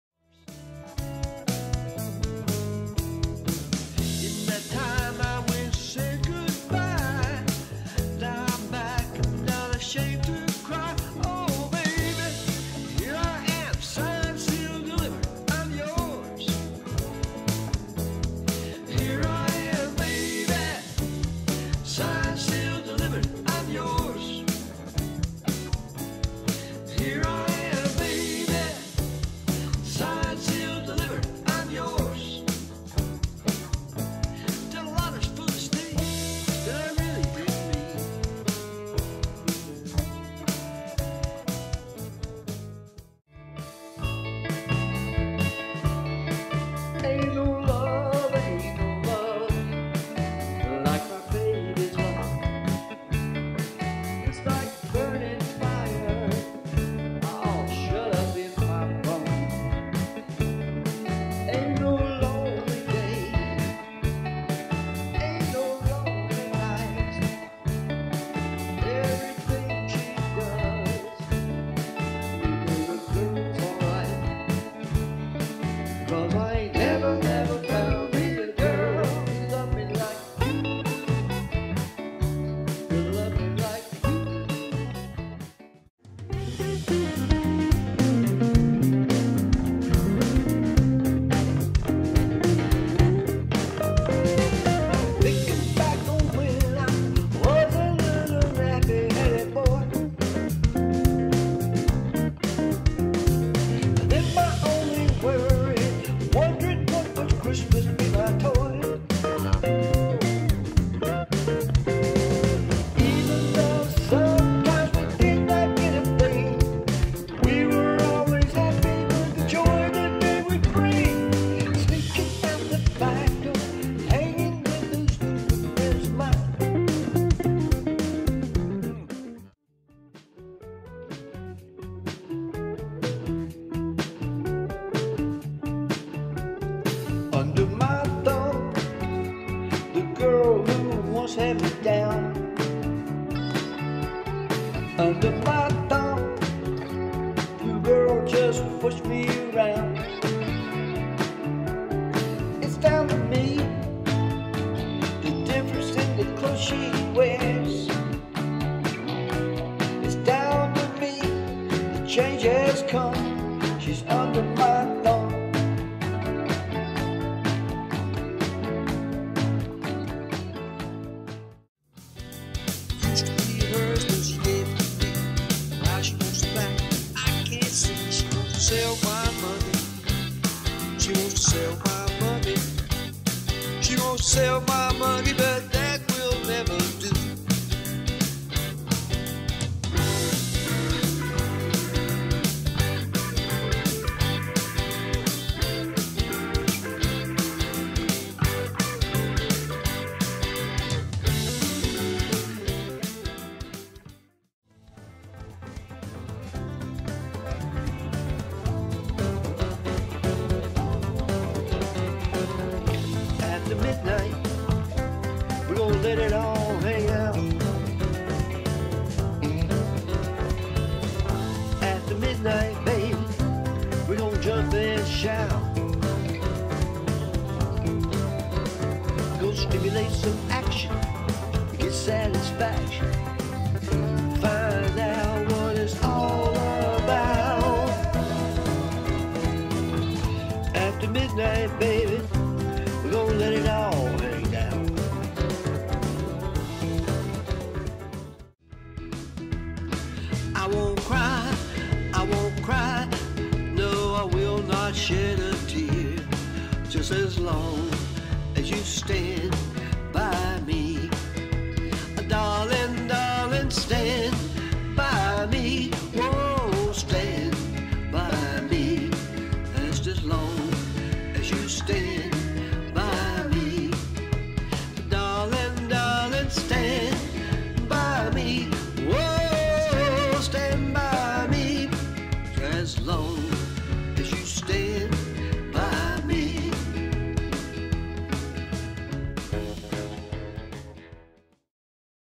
Blues Express Short Demo 2-28-19
The Blues Express Band from Winston Salem NC plays Blues Rock & Beach